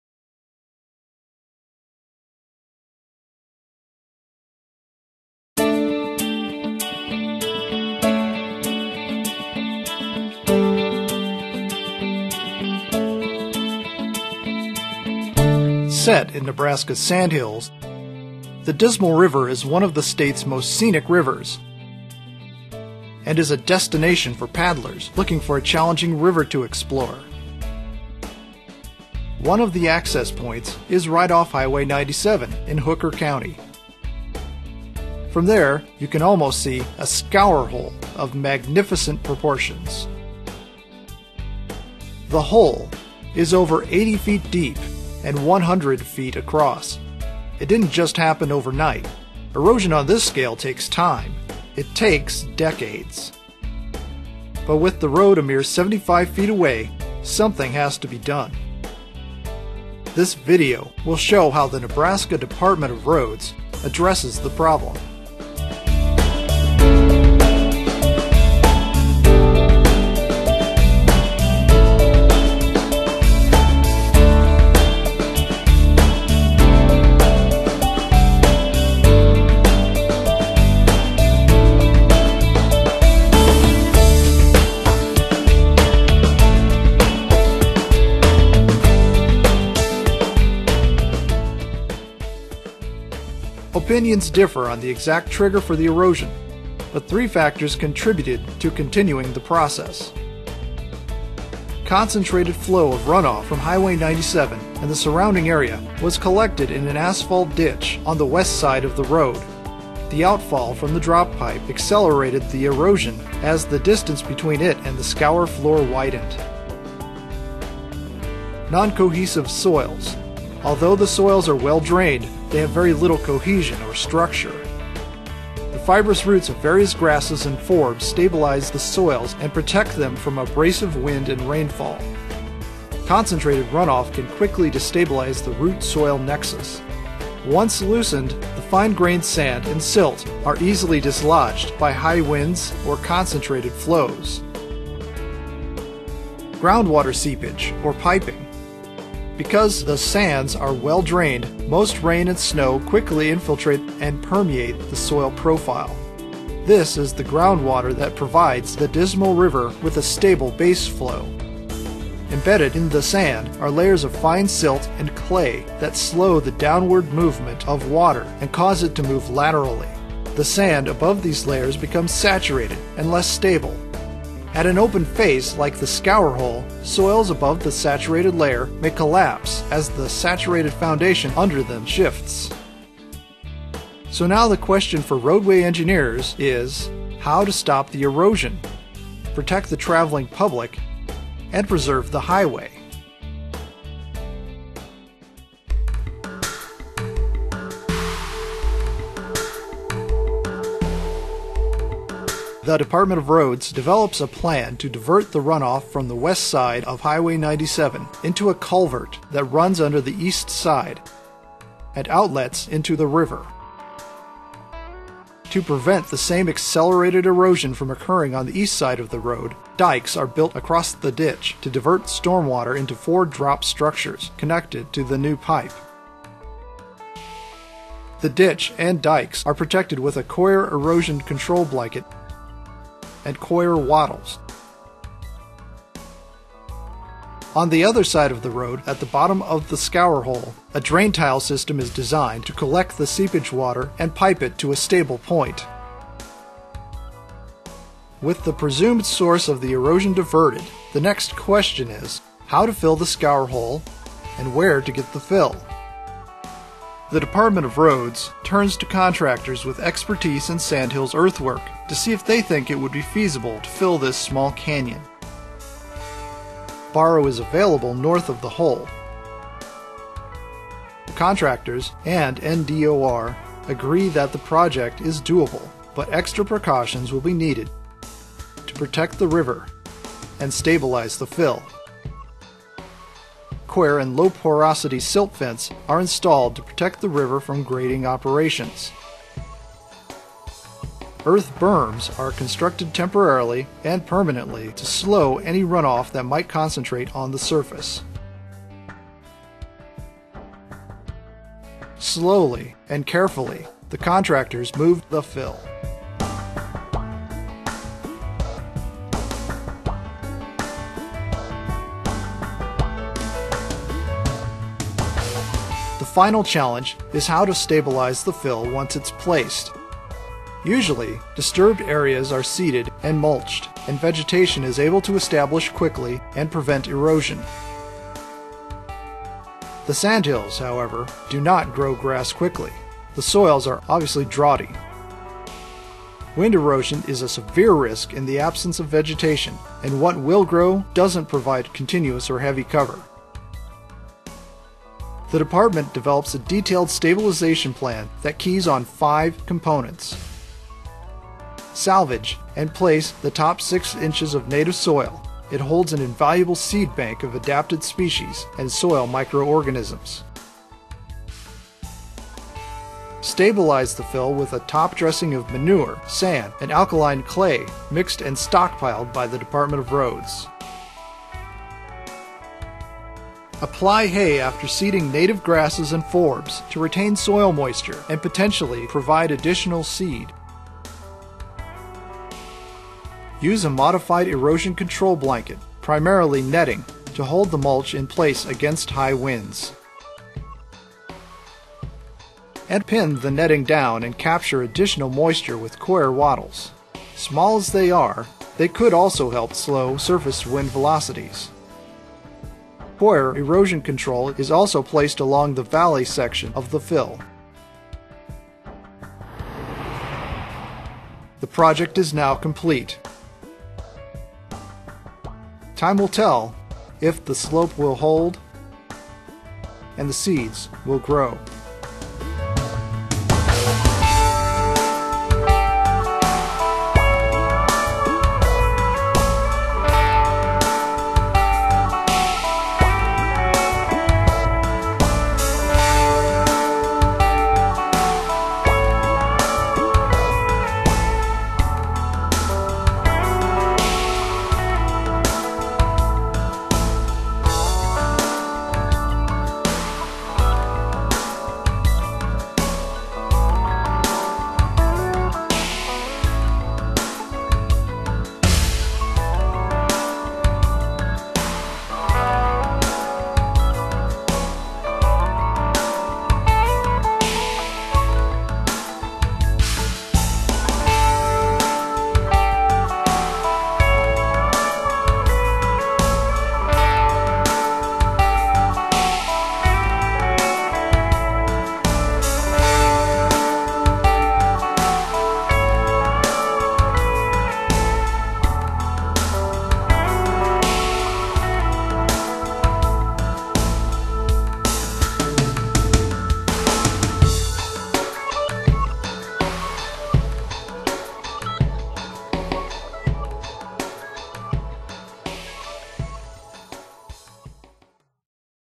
Stabilization in the Sandhills_031109a.wmv
Nebraska Department of Roads video on stablization of extreme erosion in the Sandhills by the Dismal river